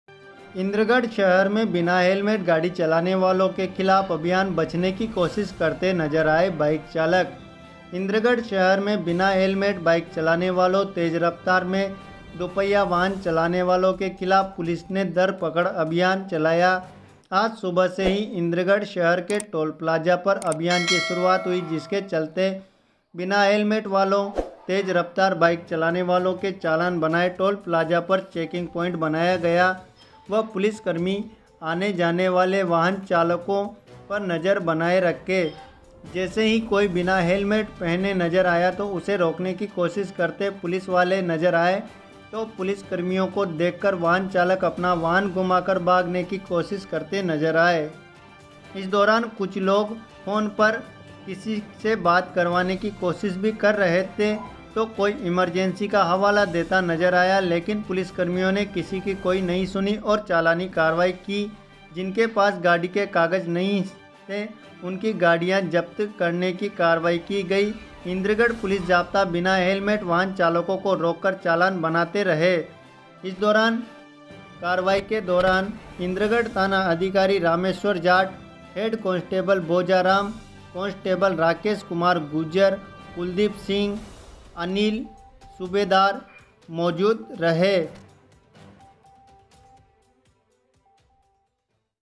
इंद्रगढ़ में हेलमेट अभियान को लेकर पुलिस ने काटे चालान, जानें कहां लगाई नाकाबंदी
toll Plaza
helmet Abhiyan